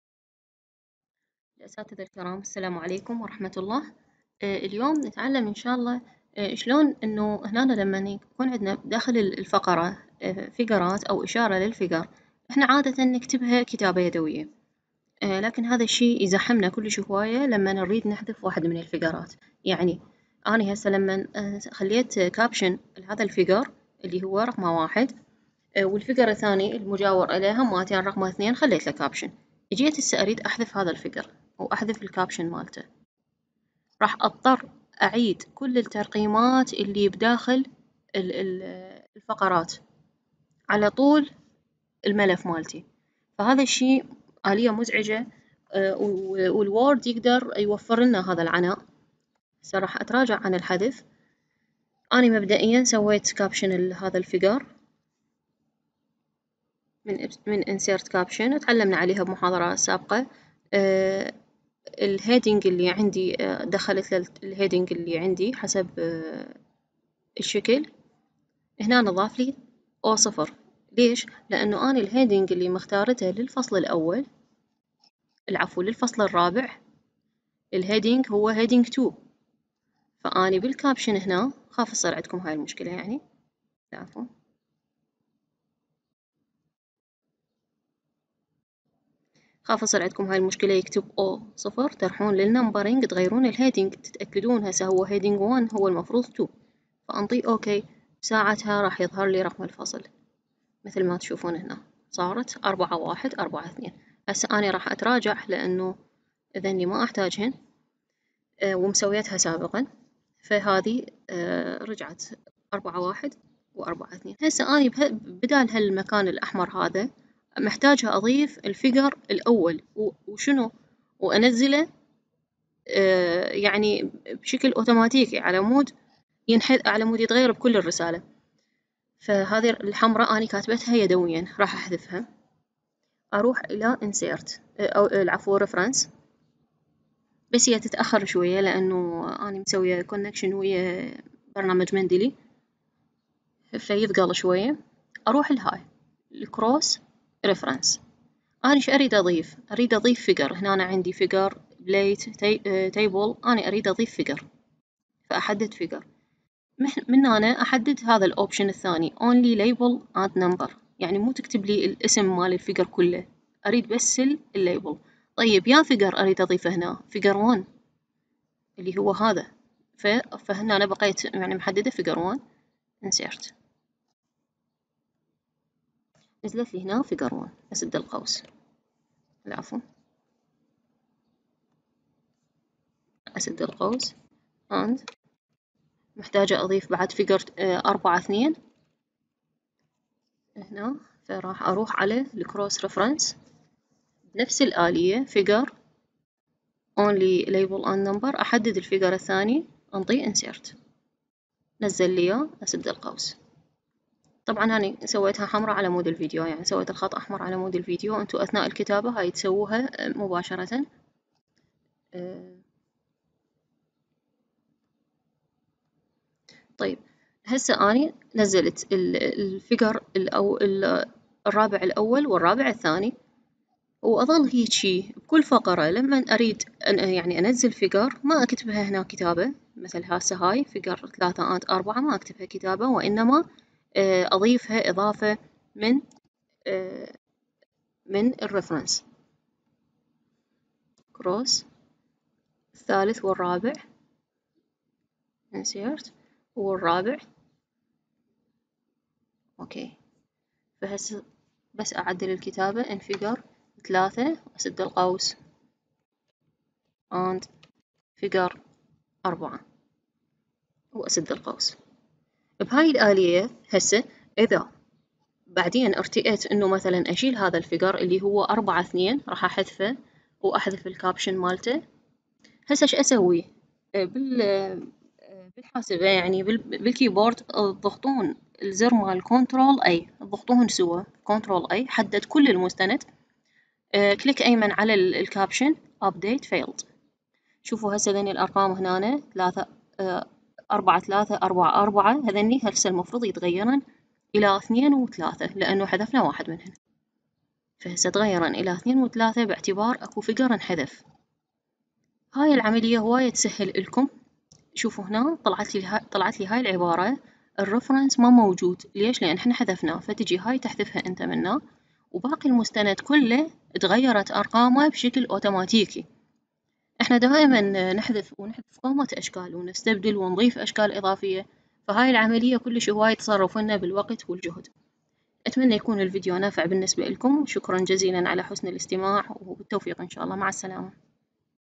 تضمين ارقام الاشكال بالنص بشكل آلي Insert Captions into paragraphs
كيفية ادراج اشارات ارقام الاشكال في المتن آليا فيي برنامج وورد